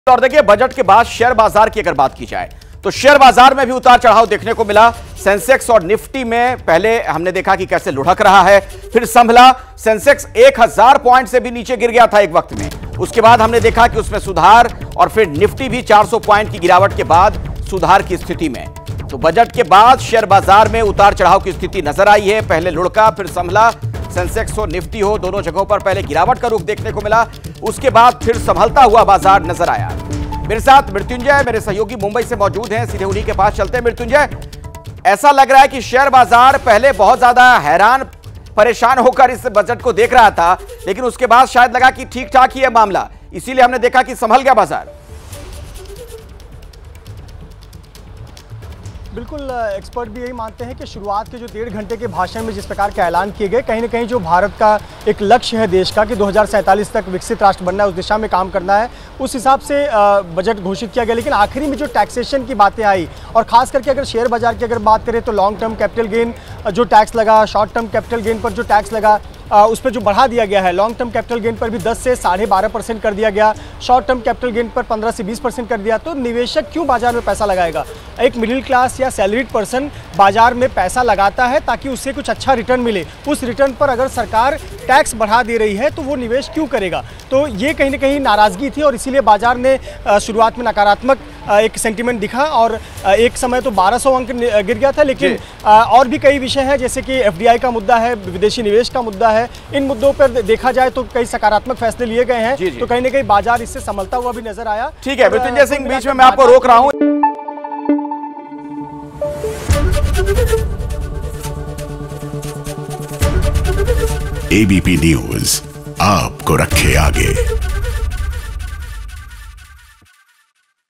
Budget 2024: बजट के बाद सेंसेक्स-निफ्टी पहले लुढ़का, फिर संभला | Stock Market Update
Budget 2024: तीसरी बार सत्ता संभालने के बाद केंद्र की नरेंद्र मोदी सरकार का पहला बजट पेश.. वित्त मंत्री  निर्मला सीतारमण ने सदन के पटल पर रखा बजट...लगातार 7वी बार बजट पेश कर बनाया रिकॉर्ड.. बजट में इनकम टैक्स को लेकर राहत न्यू टैक्स रिजीम के तहत अब 3 लाख से 7 लाख रुपए की आय पर 5% के हिसाब से टैक्स देना होगा...पहले 6 लाख तक की थी सीमा बजट पेश के दौरान सेंसेक्स में 600 अंक से ज्यादा की गिरावट...निफ्टी भी 180 अंक गिरा...दोपहर बाद सेंसेक्स ने की रिकवरी